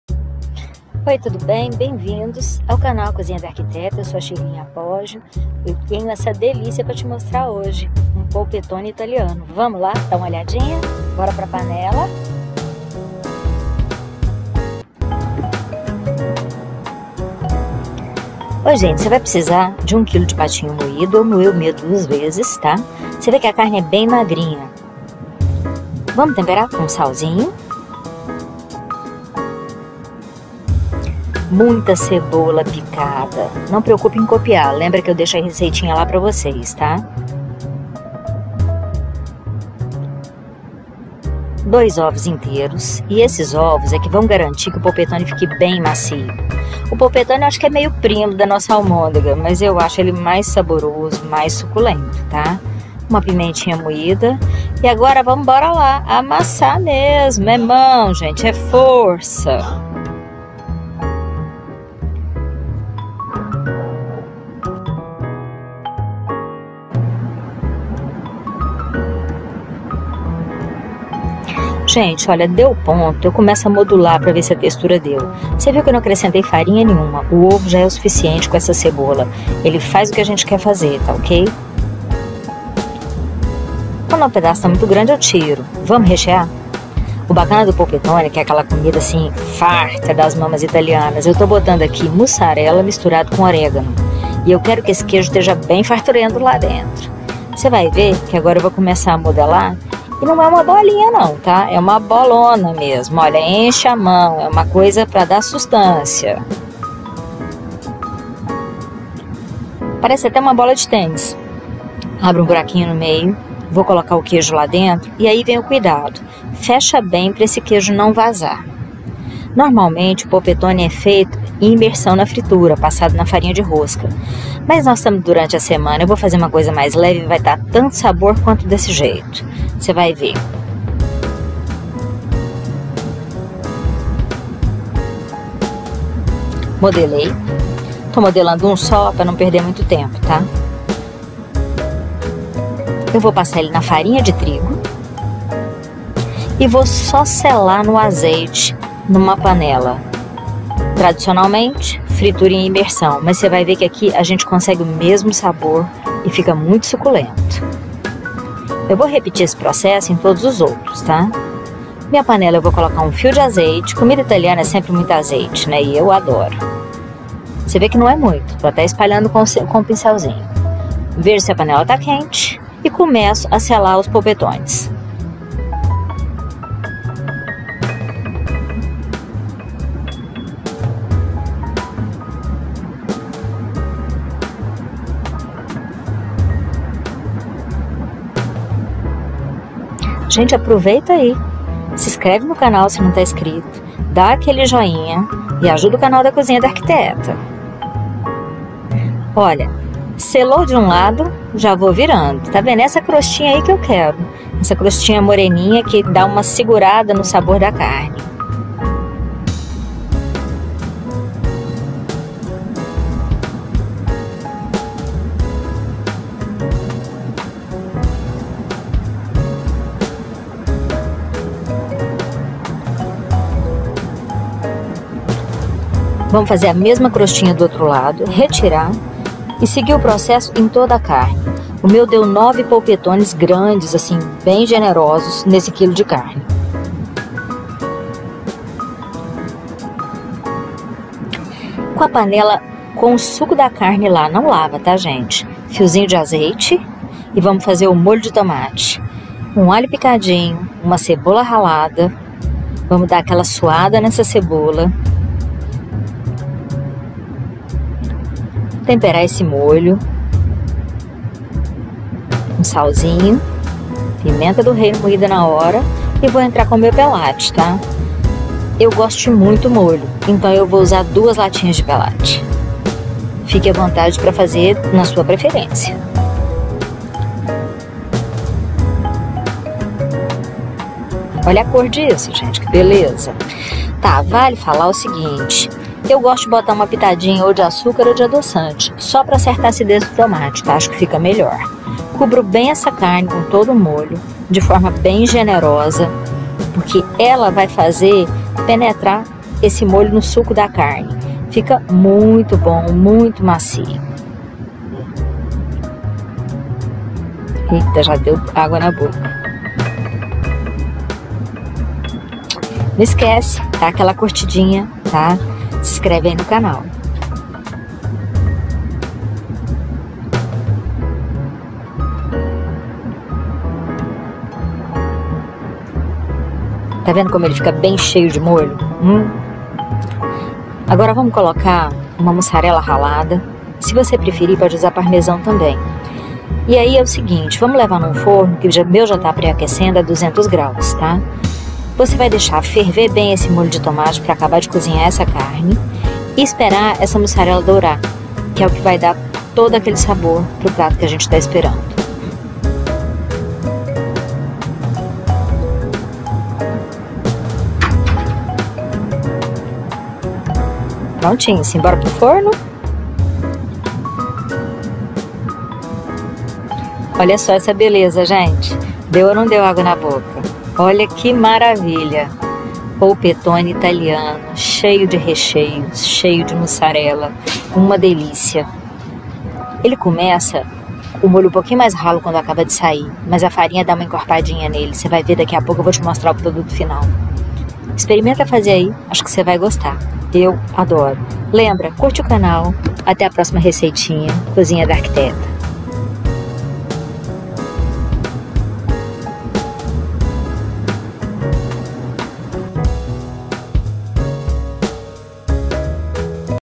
Polpetone
Hoje tem Polpetone Italiano na Cozinha da Arquiteta, super suculento e cheio de sabor, espero que vocês gostem!

Canal: A Cozinha da Arquiteta

POLPETONE ITALIANO

Ingredientes
Para o Polpetone
1 kg de patinho moído 2 vezes
1 cebola ralada ou processada
1 colher de sopa de sal
Pimenta do reino moída na hora
2 ovos
Farinha de trigo o quanto baste para empanar
3 colheres de azeite para selar os polpetones
2 xicaras de queijo muçarela ralado ou processado (1 para o recheio e a outra para gratinar)
1 colher de sopa de orégano

Para o Molho Pomodoro
2 latas de tomate pelati
1 cebola ralada
1 colher de sobremesa de sal (corrija se necessário)
3 colheres de azeite
1 pitada de açúcar
Modo de fazer descrito no vídeo